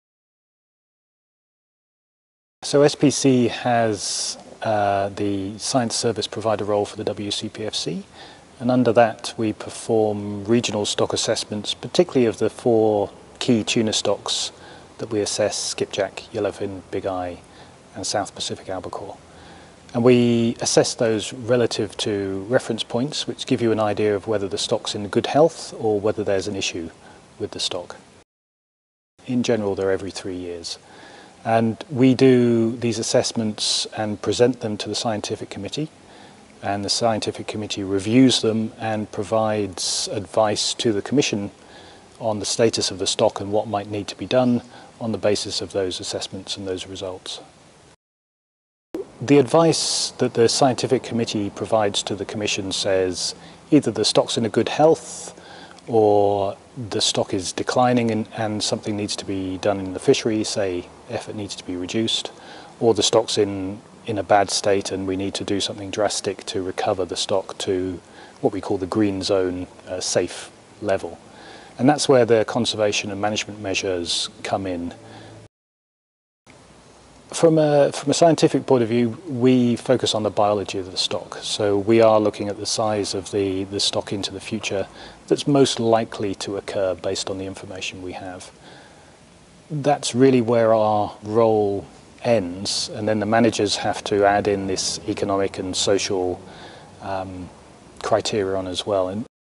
Graham Pillings - SPC stock assessment
Graham Pillings explains SPC’s role in conducting stock assessments on the 4 major tuna species in the Western and Central Pacific, and how that feeds into decisions made by the WCPFC.